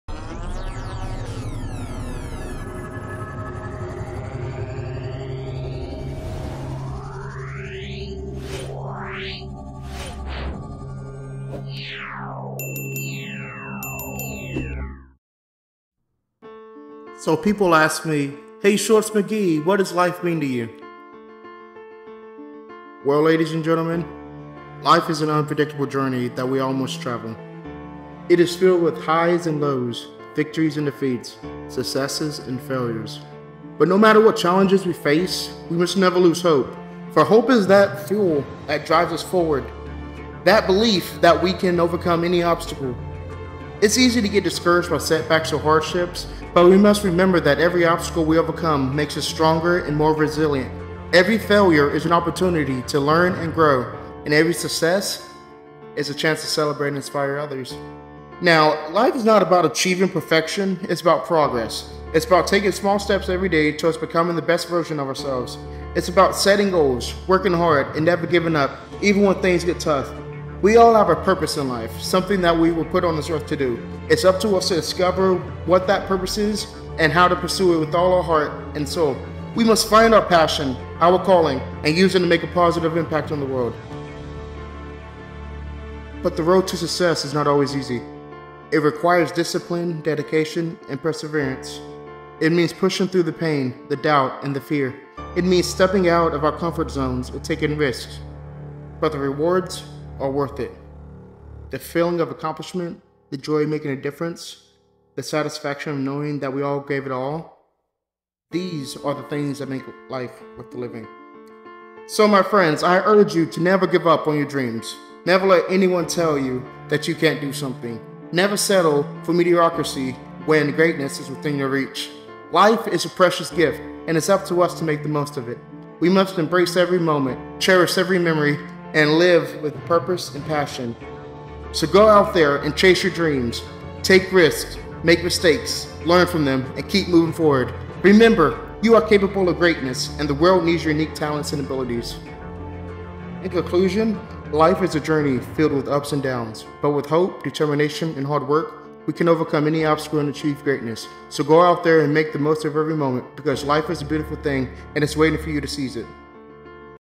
Life
Ladies and gentlemen, life is an unpredictable journey that we all must travel. It is filled with highs and lows, victories and defeats, successes and failures. But no matter what challenges we face, we must never lose hope, for hope is the fuel that drives us forward, the belief that we can overcome any obstacle.